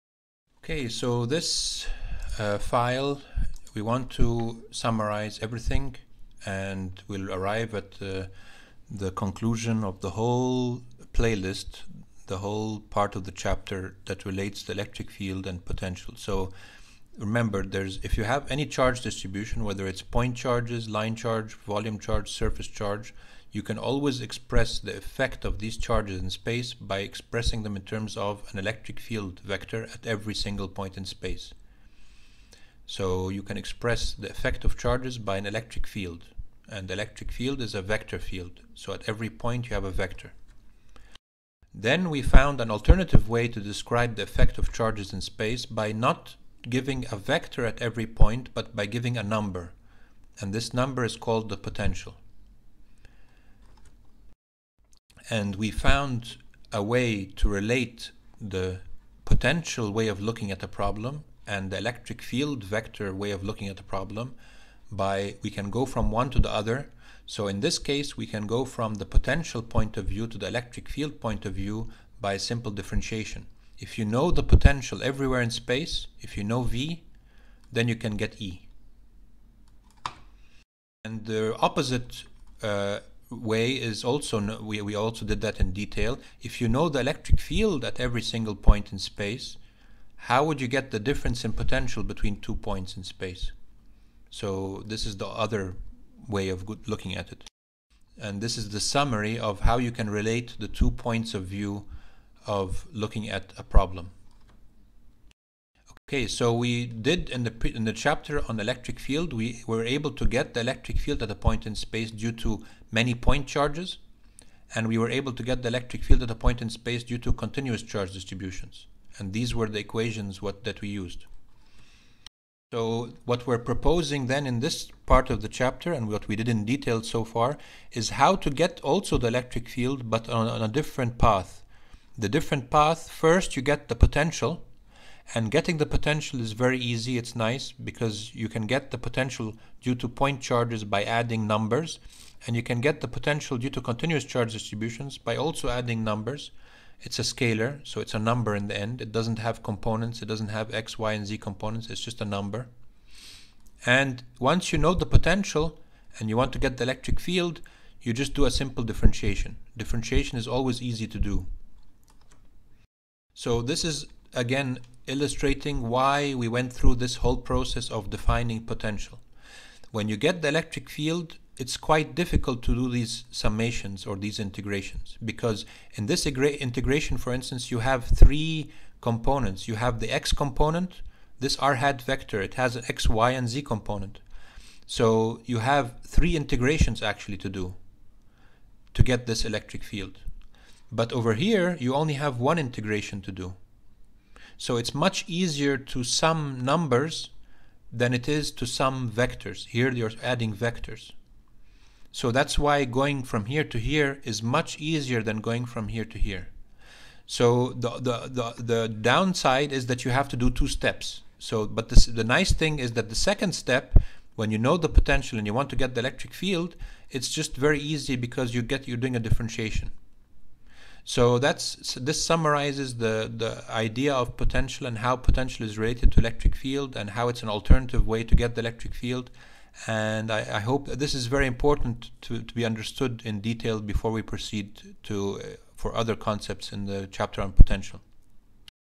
5 grand summary
summary of the concept of potential and how it is related to the electric field